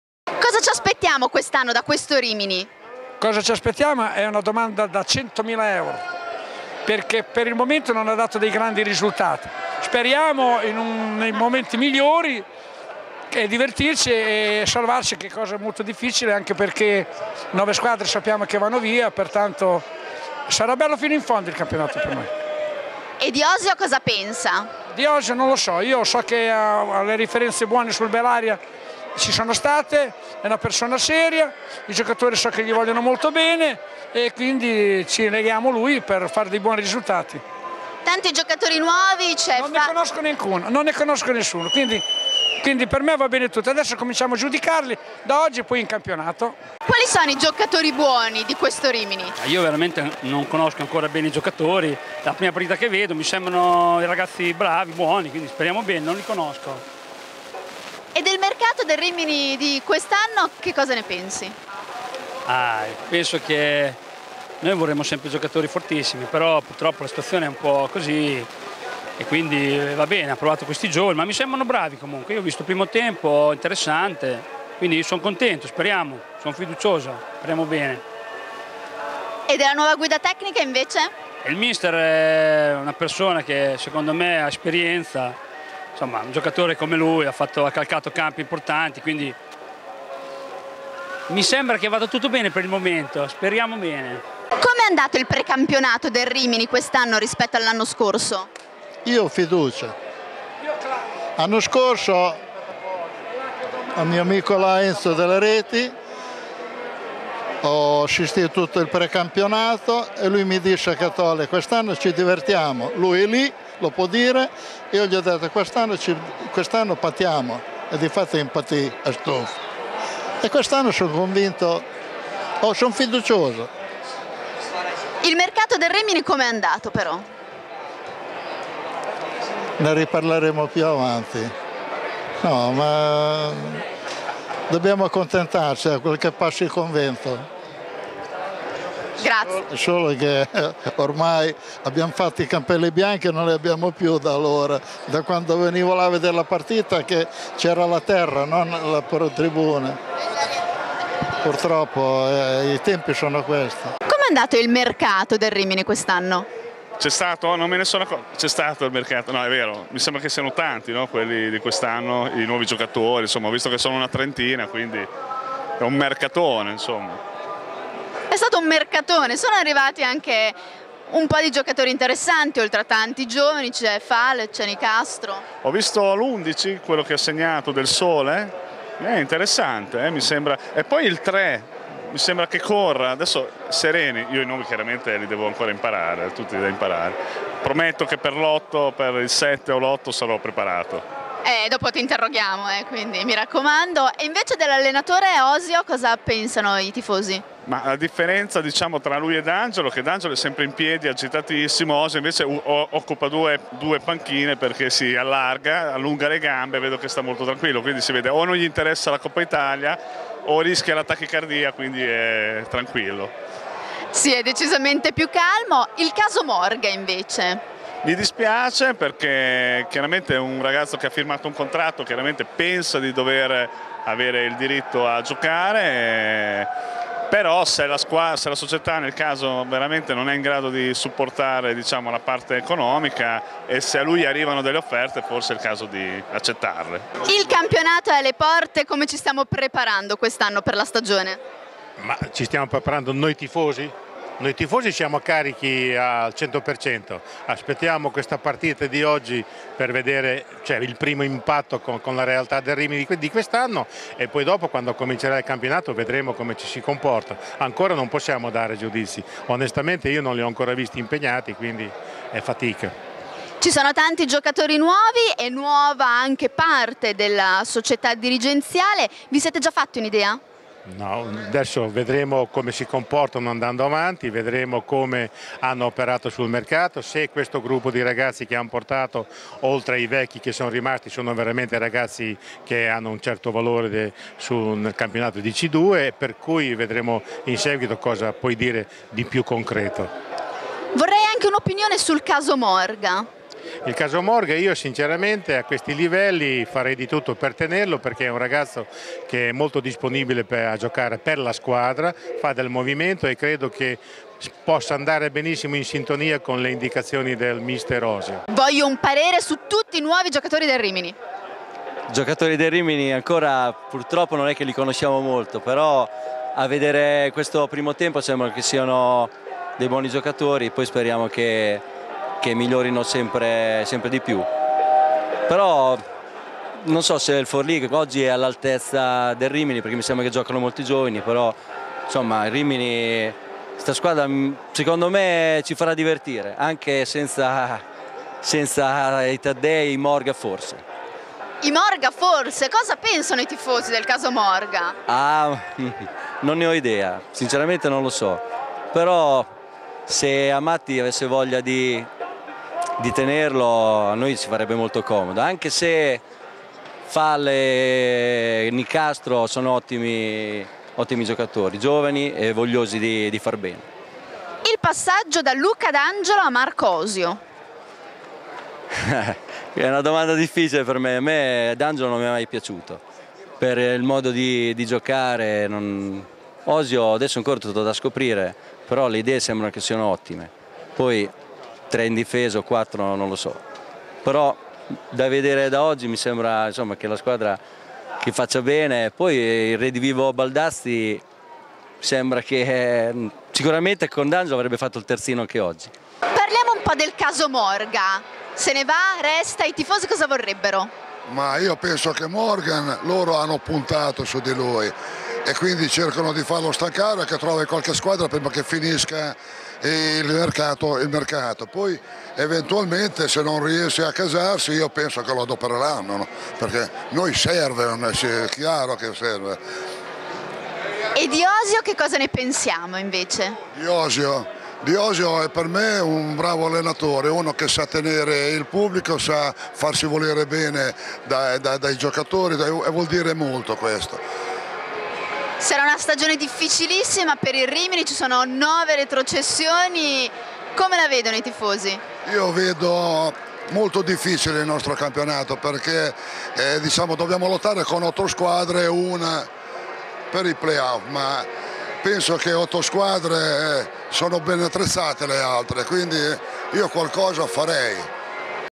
Icaro Sport. Rimini Calcio, la parola ai tifosi
Interviste realizzate da Genny Bronzetti in occasione del match di Coppa Italia Rimini-Forlì del 28 agosto 2013.